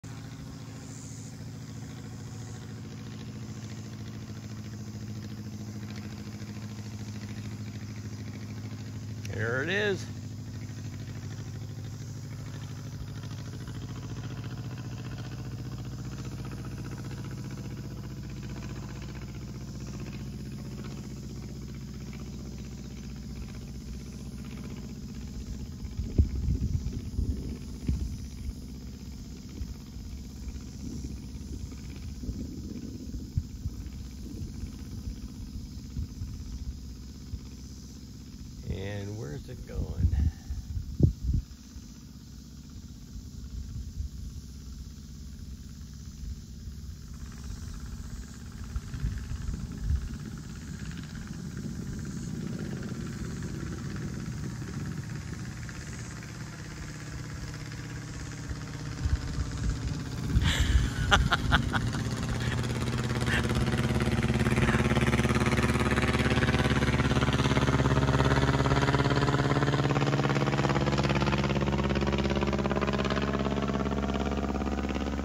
Pietenpol Bombing Run
My buddy practicing his bombing TP skills.